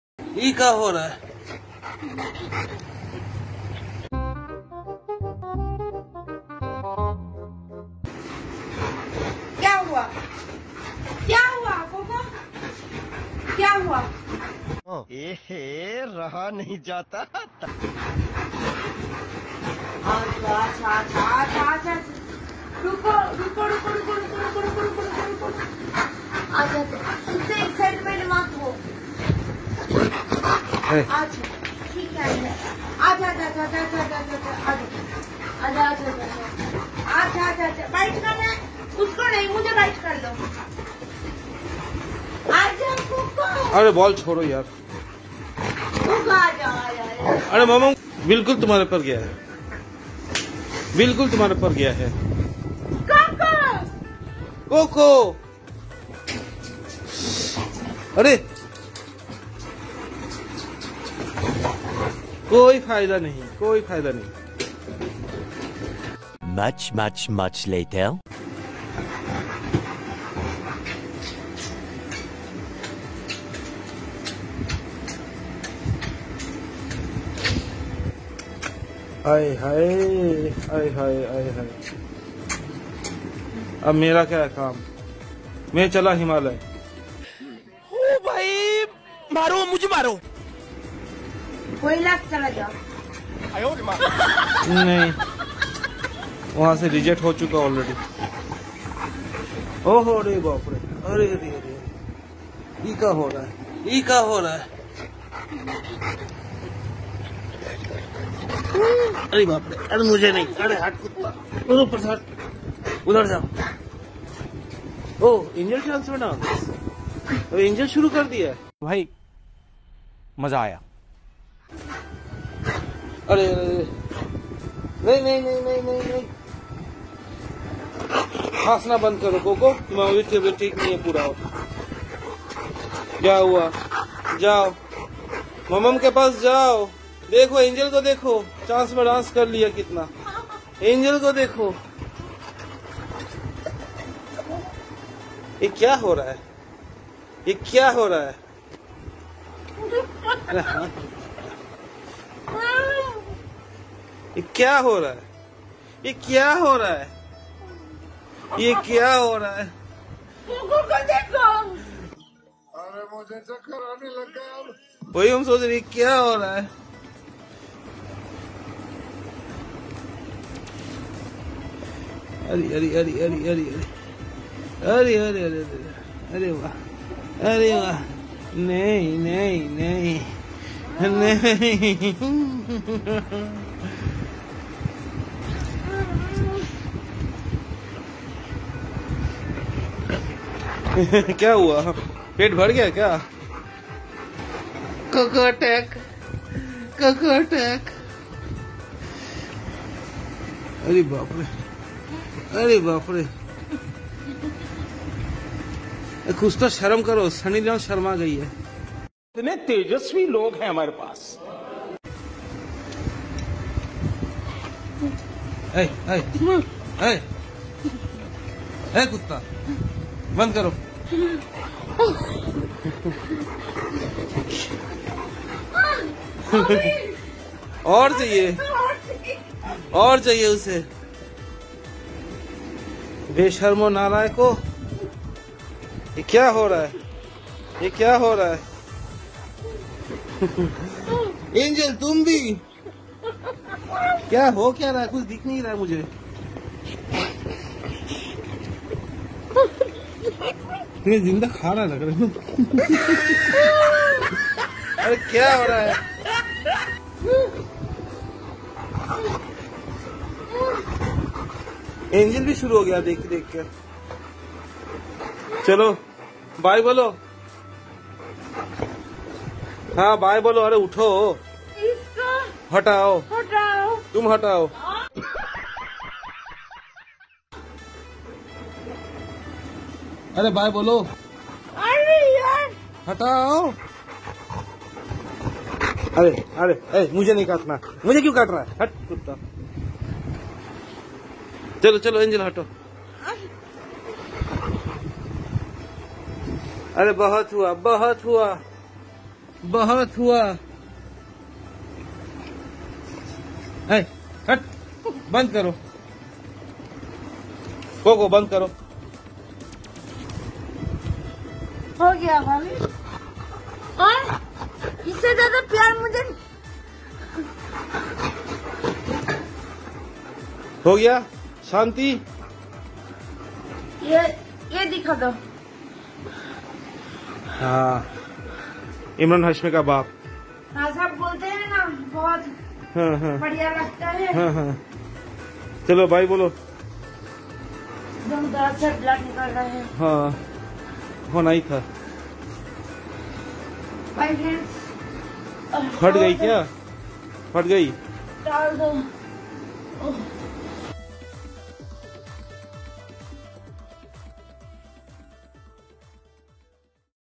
Watch and Enjoy the video, where Coco loves Mumunconditionally. Their love is exceptional...😍😍😍😍
Coco and Angel loves their Mum very, watch what
happens, when Mum returns from adate night with Dad.
watch the fun loving video till the end. 🤣😘🤣😘🤣😘🤣😘🤣😘🤣